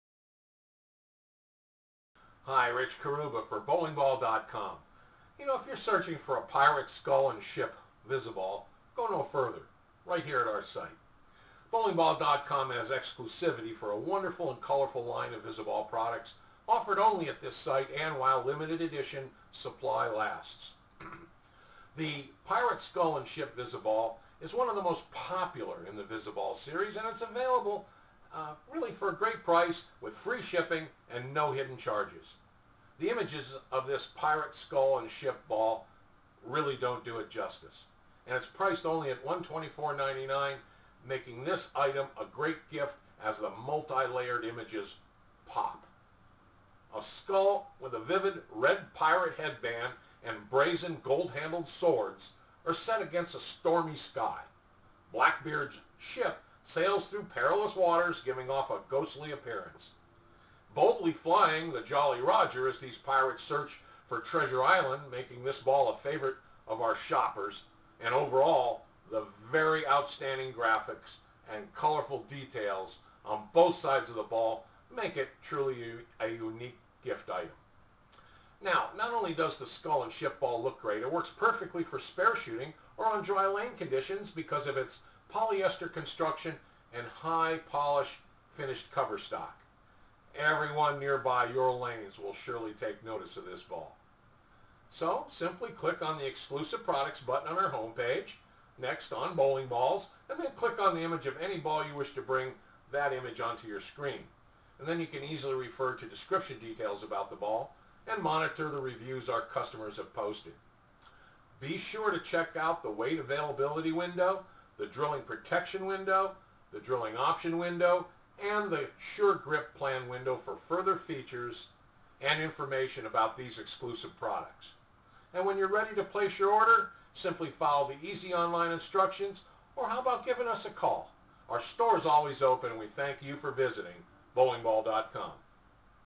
Pirate Skull & Ship Viz-A-Ball
Ahoy Mateys! Set said with Rich Carrubba as he tells you all about the bowlingball.com Exclusive Pirate Skull & Ship Viz-A-Ball!